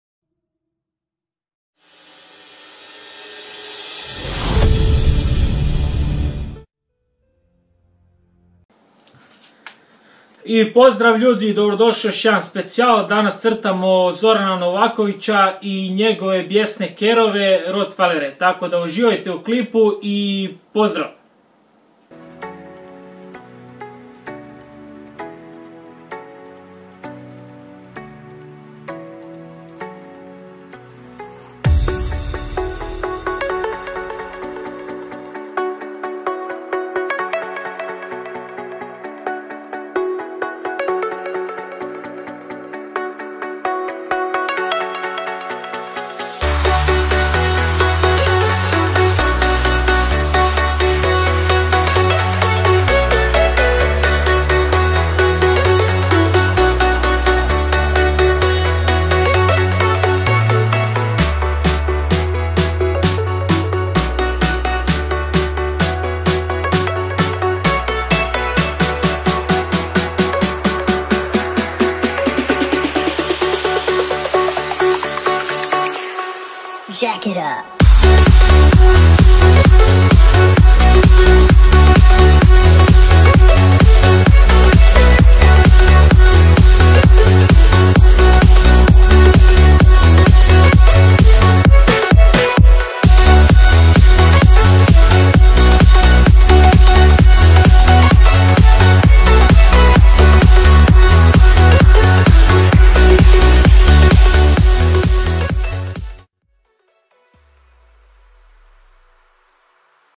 zRN1337 Rottweiler Dogs Drawing - Zoran Novaković Crtež
I always do sketch first because video would be too long.
Tutorial soon on my channel

Paper size : 50*35 cm
Technic : markers
Real time : 2 hours